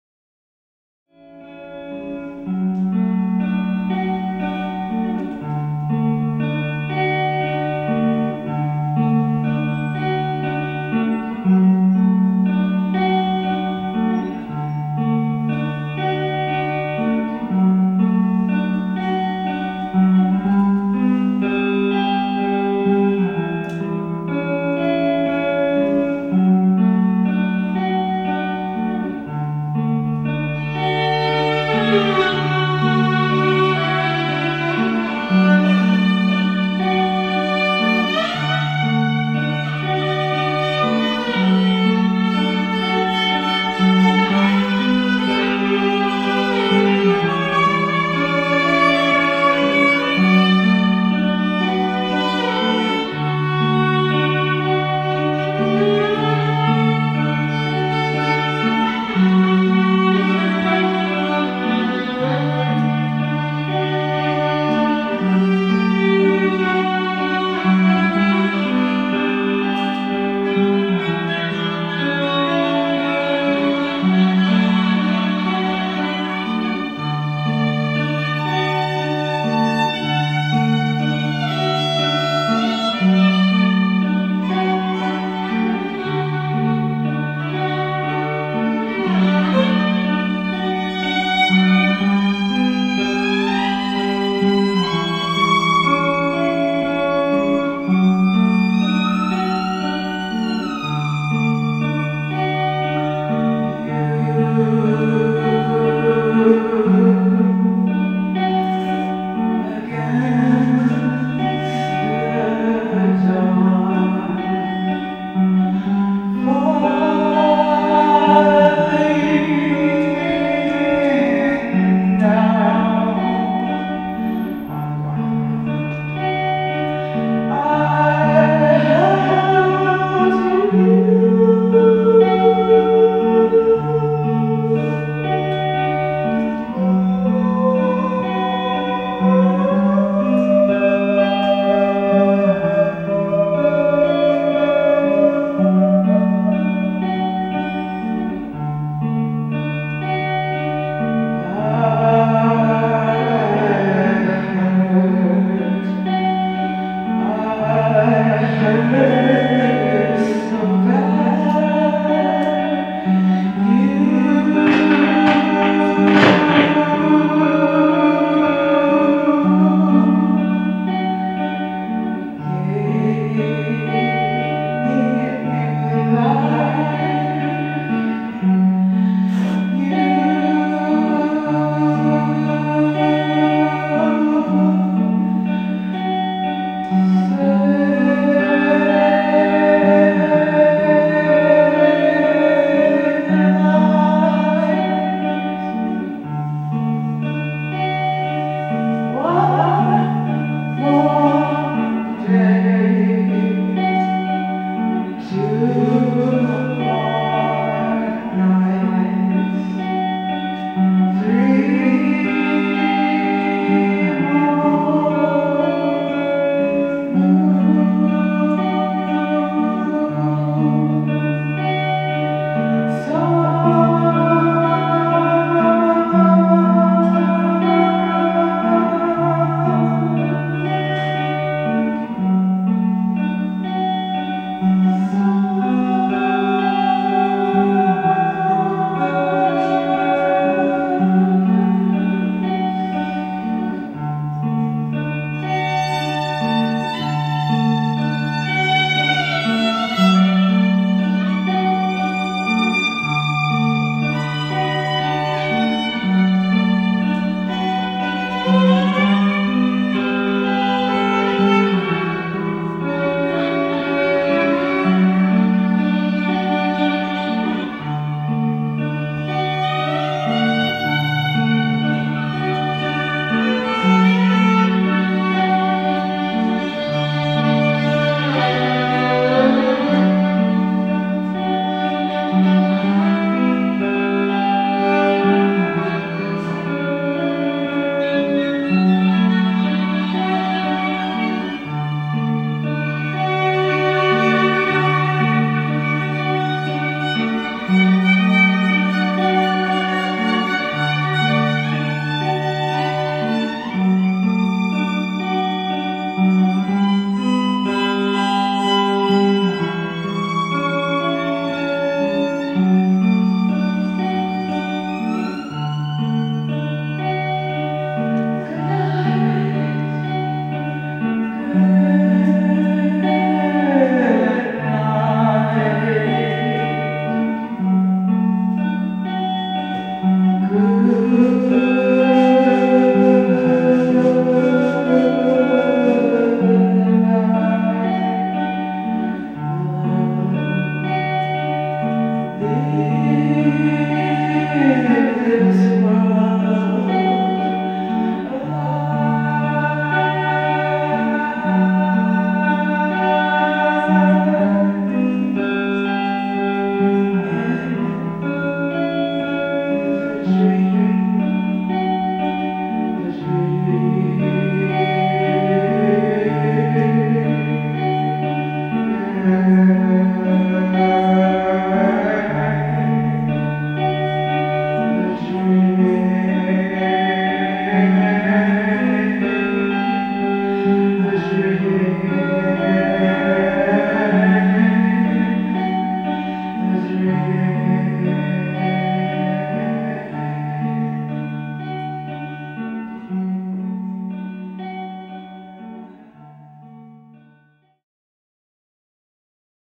Dust Dive Flash/Laura Ortman @ Eternal X-Mas Pt.3
Dust Dive/Laura Ortman live @ Eternal Christmas - Brooklyn, NY - December 10, 2009.